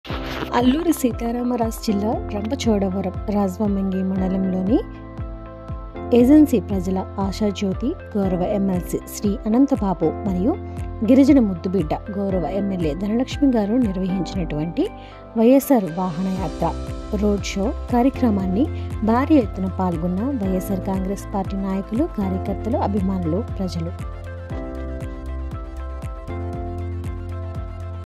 రాజవొమ్మంగి: వైయస్ఆర్ వాహన యాత్ర
ఈరోజు అల్లూరి సీతారామరాజు జిల్లా రంపచోడవరం రాజవొమ్మంగి మండలంలో మన ప్రియతమ నాయకుడు ఏజెన్సీ ప్రజల ఆశాజ్యోతి గౌరవ MLC శ్రీ అనంత బాబు  మరియు మన గిరిజన ముద్దు బిడ్డ గౌరవ MLA ధనలక్ష్మి గార్ల నిర్వహించిన
వైయస్ఆర్ వాహన యాత్ర(రోడ్ షో) కార్యక్రమానికి భారీ ఎత్తున పాల్గొన్న వైఎస్ఆర్ కాంగ్రెస్ పార్టీ నాయకులు కార్యకర్తలు అభిమానులు ప్రజలు